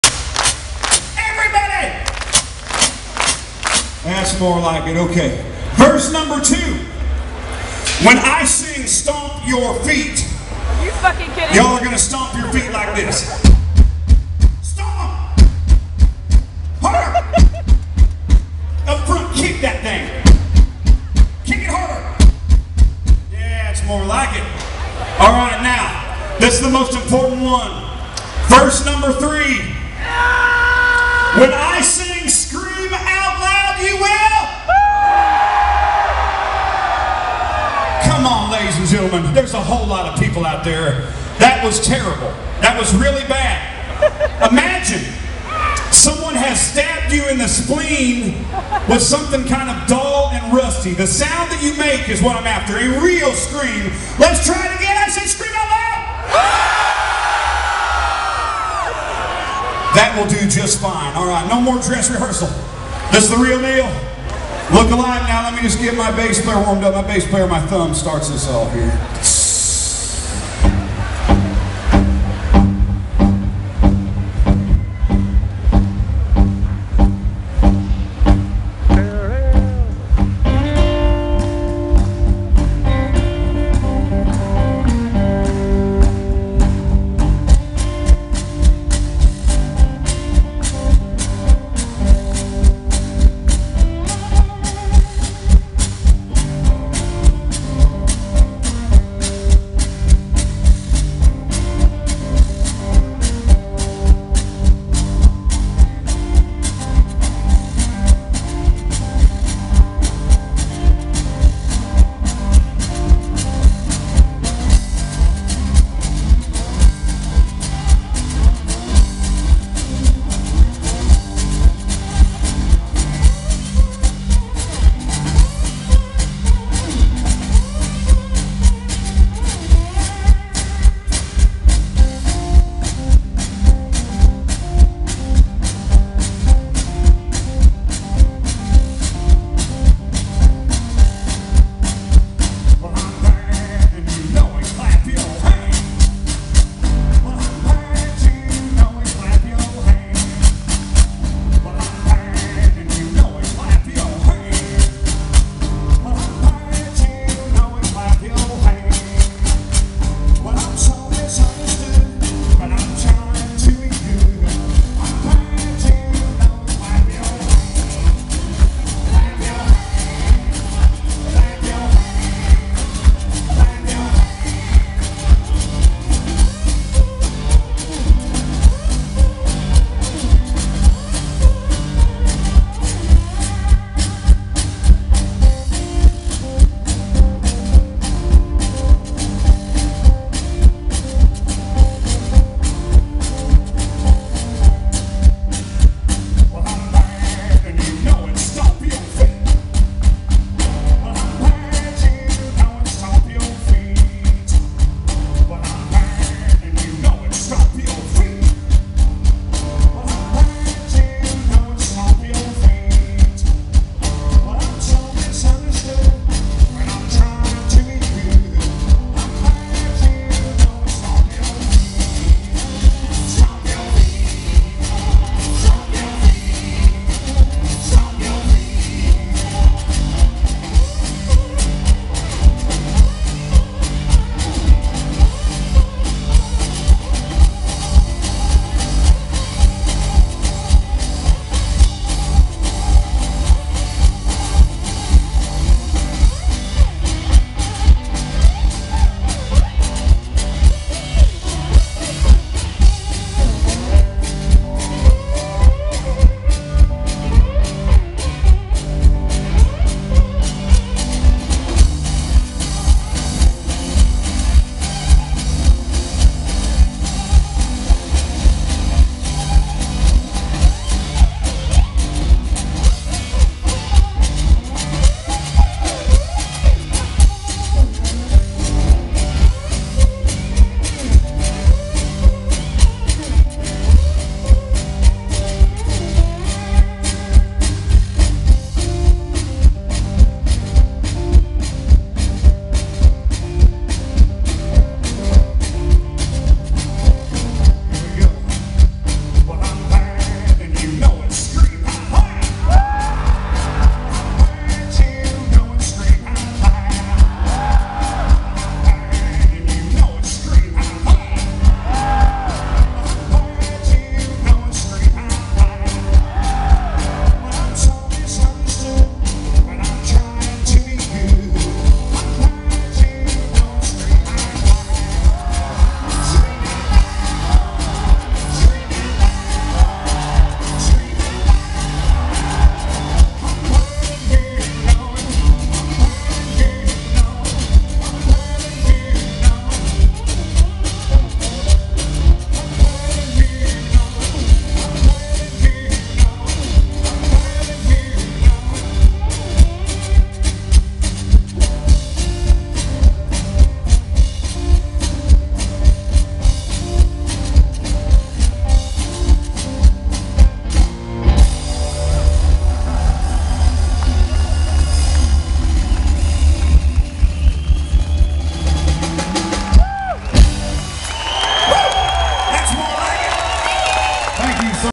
Reverend Peyton's Big Damn Band at Parkpop 2016
We enjoyed every tune and every word this performance.
Stomp your feet!!... In this mud?¿ You gotta be F* kiddin' me! Hahahaha!